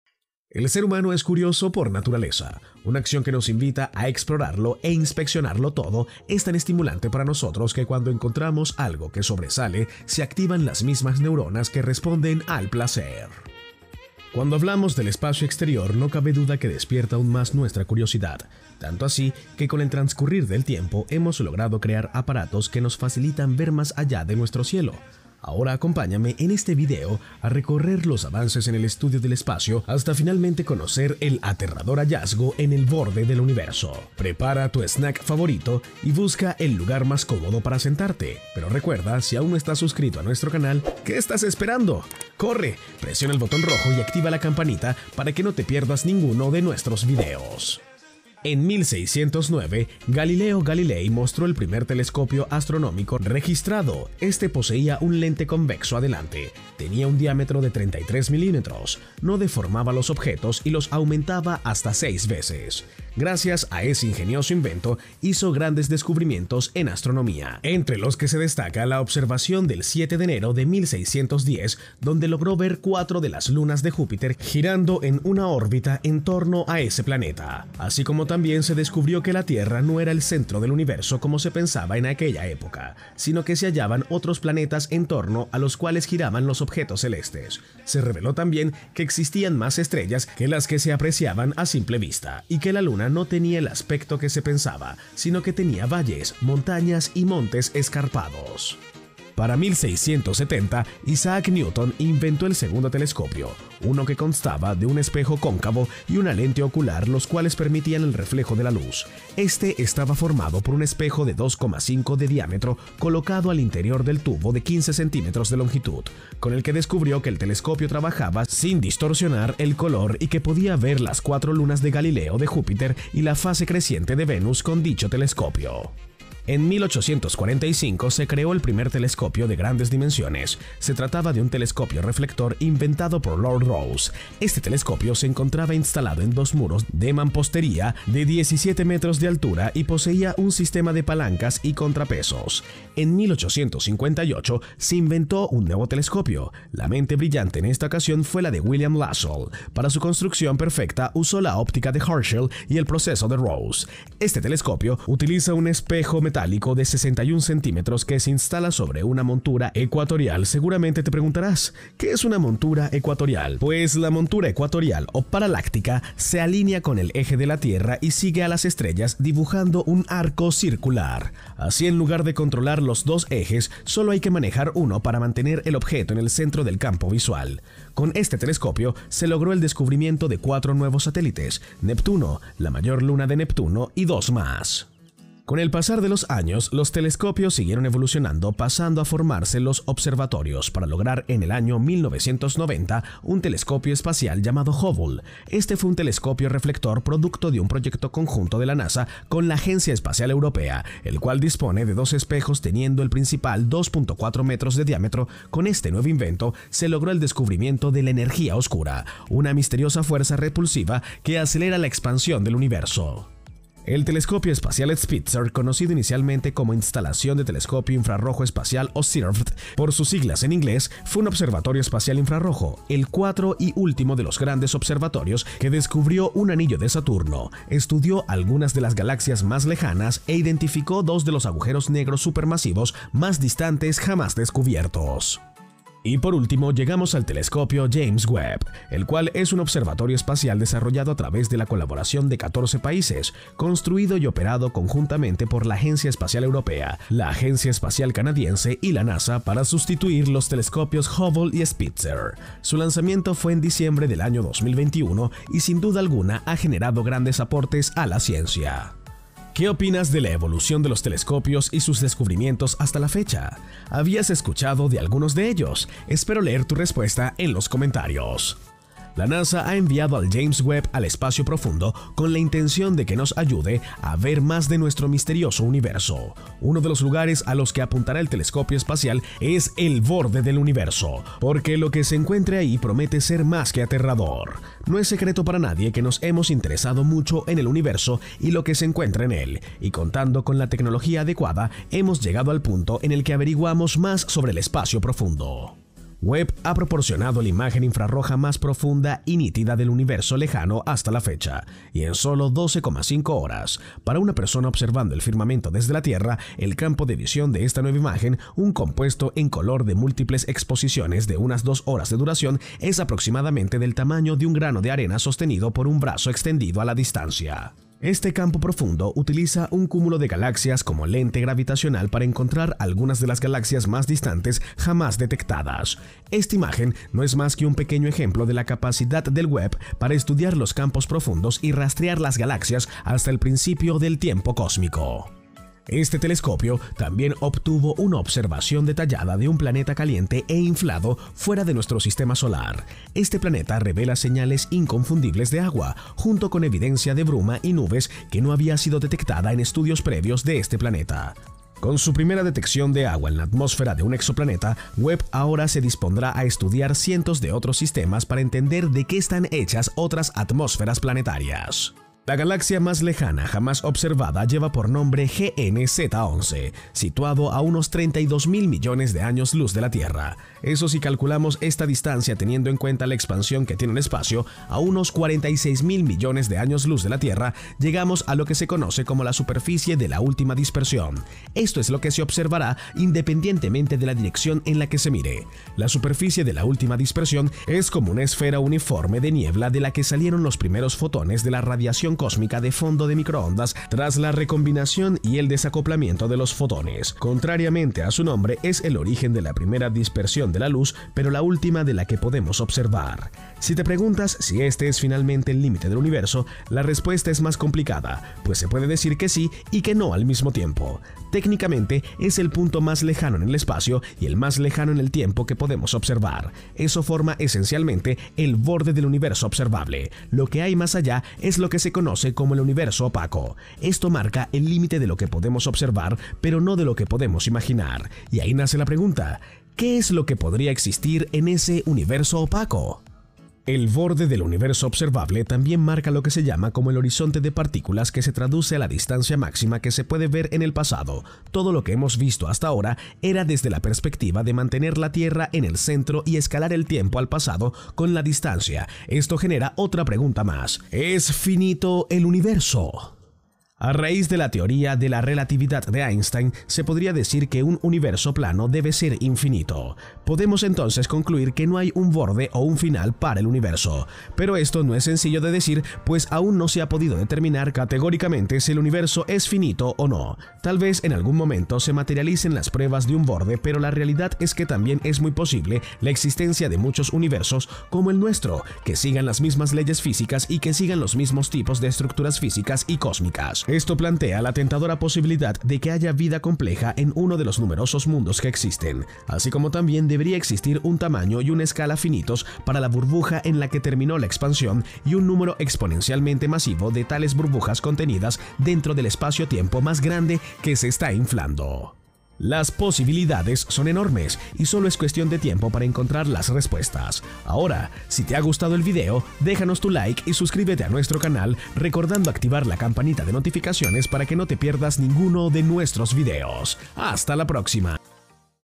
¡El telescopio James Webb hace un descubrimiento aterrador en el borde del universo!
💵¿TE HAS IMAGINADO CÓMO SERÍA CONVERTIRTE EN MILLONARIO EN POCOS AÑOS?

¿Estás listo para descubrir los secretos del universo? En este video, te mostraremos cómo el Telescopio James Webb hizo un descubrimiento aterrador en el borde del universo. Síguenos mientras exploramos los misterios que se encuentran más allá de nuestra comprensión y echa un vistazo a lo que está por venir. ¡Mira cómo el Telescopio Webb nos ha ayudado a descubrir algunos de los misterios del espacio y cómo este conocimiento puede ayudarnos a expandir nuestros horizontes!